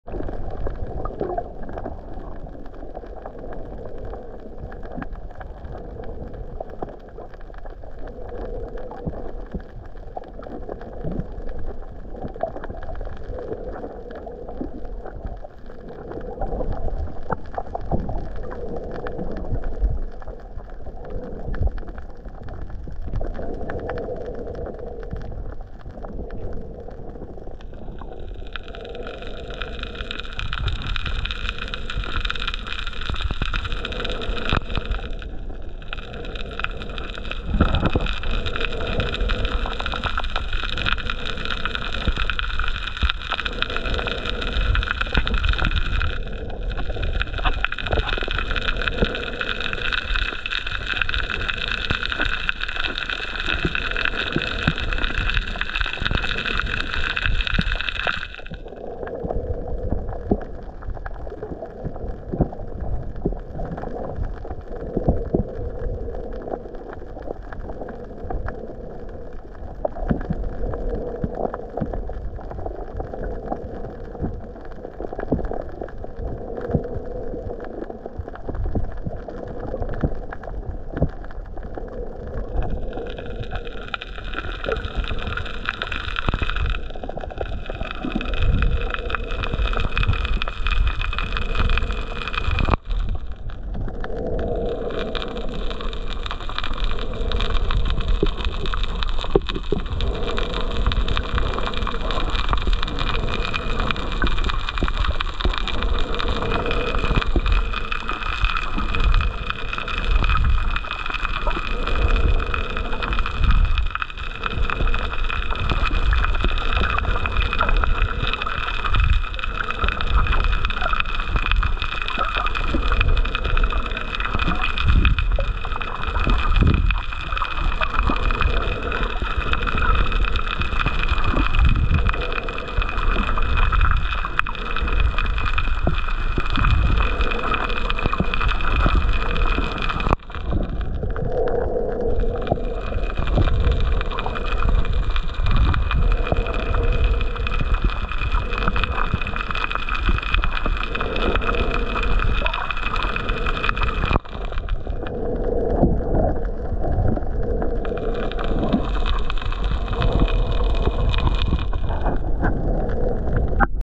Snorkeling with turtle around Gili Trawangan
Snorkeling with a turtle around Gili Trawangan,  Lombok,  Indonesia. With a nice little surprise at the end.